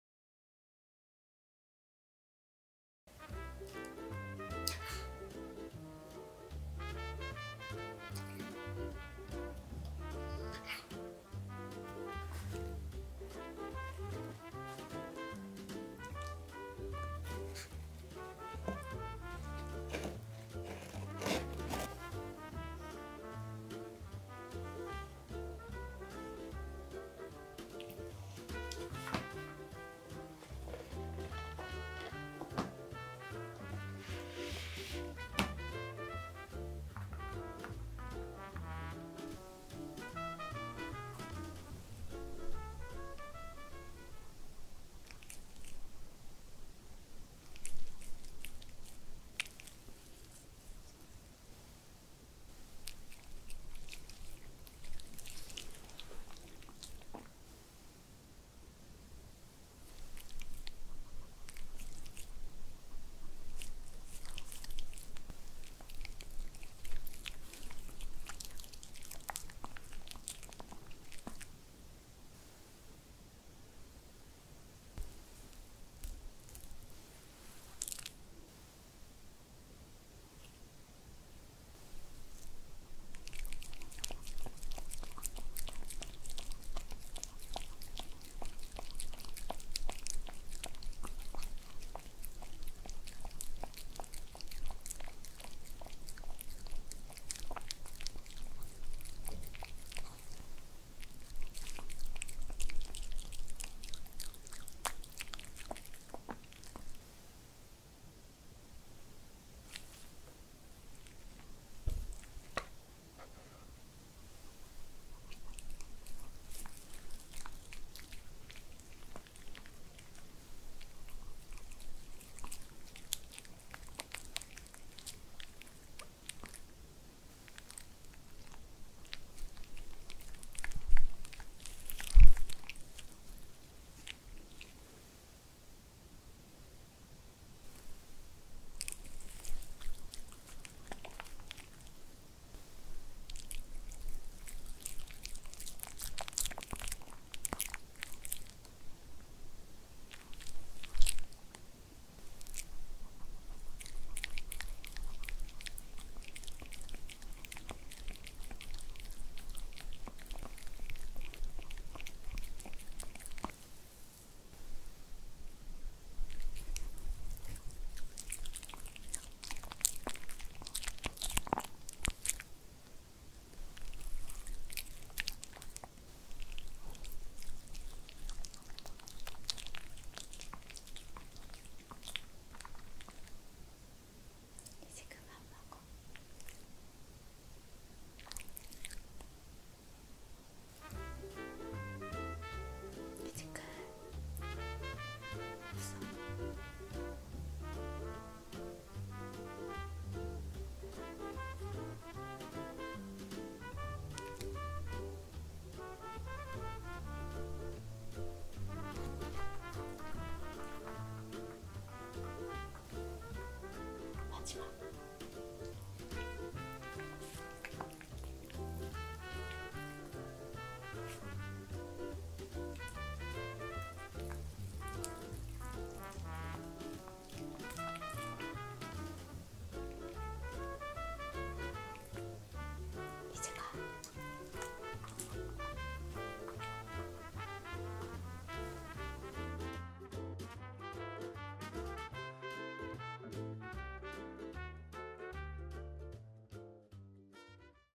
썸타는 강아지의 캐롭으로 만든 초콜릿 디저트 먹방 eatingASMR (강아지 먹방) DOG CAKEPOPS, CAROB CHOCOLATE FOR DOGS MUKBANG
발렌타이데이라서 설이 여자친구한테 줄
강아지용 캐롭초콜릿을 만들어봤어요.
여친한테 주기전에
설이가 먼저 맛보는 먹방asmr 입니다!! ㅋㅋㅋㅋㅋ

진짜 초콜렛이 아닌 대부분 고구마와 닭고기로 만든 고기빵(?)에 ㅋ한천과 캐롭을 뿌려만든 강아지용 음식이랍니다:)

그럼 오늘도 즐겁게 시청해주시고
사용한 모든 재료는 영상을 참고해주세요!!

Profile💛
Name : Hong seol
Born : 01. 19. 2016
Breed : White Pomeranian(♂ )
Favorite Food : Yogurt&Cheese